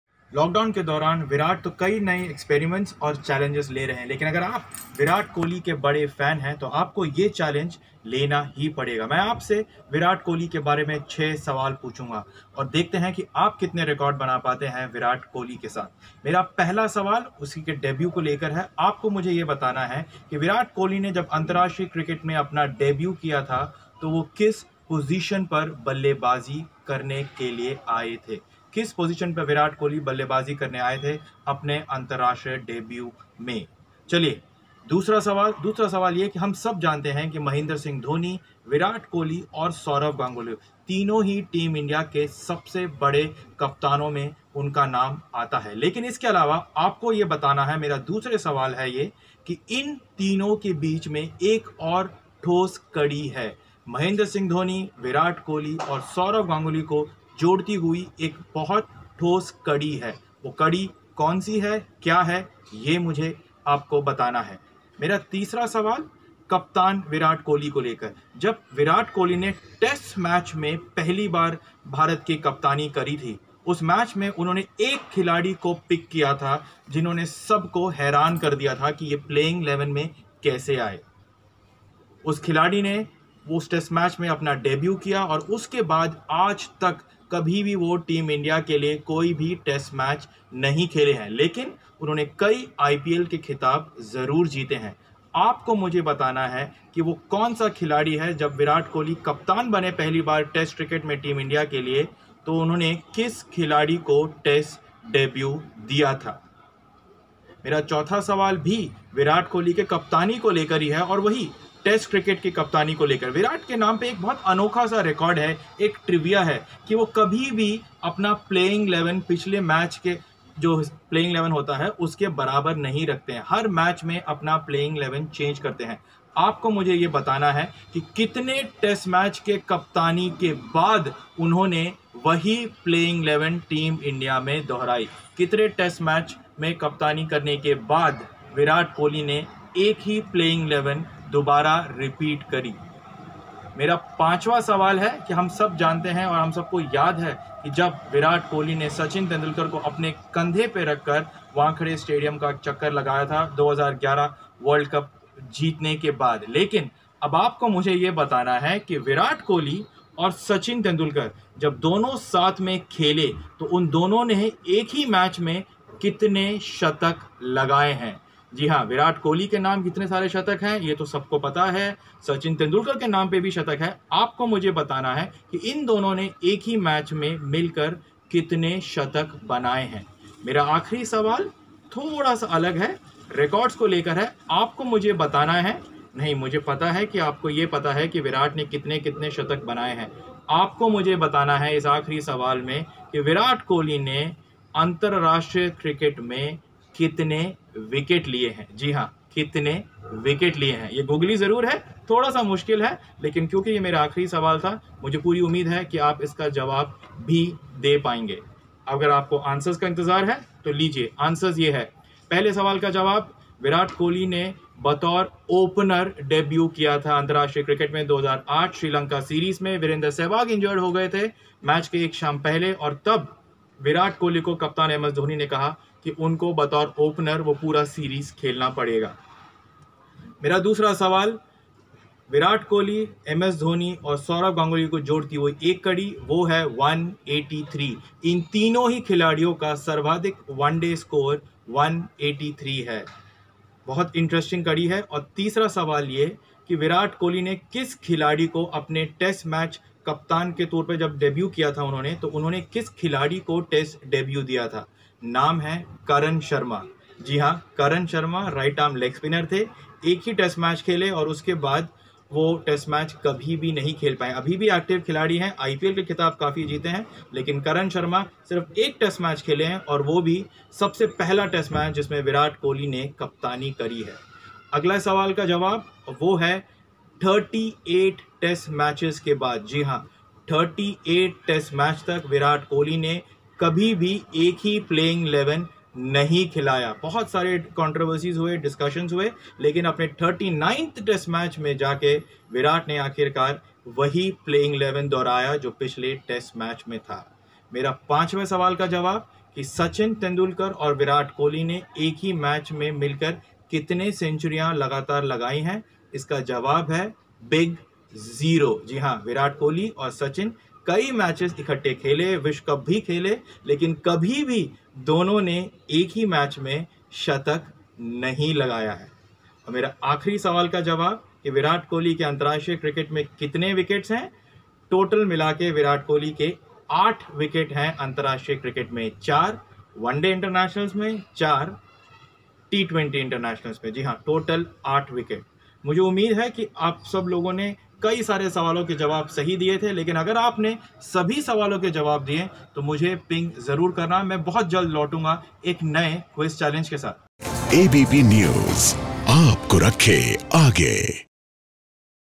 Virat Kohli Challenge: अगर हैं विराट कोहली के सुपर फैन तो दीजिए इन सवालों के जवाब
21 दिनों तक पूरे भारत में लॉकडाउन किया है. कोई भी अपने घर से बाहर नहीं निकलेगा. इसका अहम मकसद कोरोना वायरस को खत्म करना है. ऐसे में लोग घरों पर ही हैं. इसी को देखते हुए हम आपके लिए एक सेगमेंट लेकर आए हैं जहां आपको सिर्फ 6 सवालों के जवाब देने हैं जो भारतीय क्रिकेट टीम के कप्तान विराट कोहली से जुड़े हुए हैं. अगर आप हैं विराट कोहली के फैन तो दीजिए इन आसान सवालों के जवाब.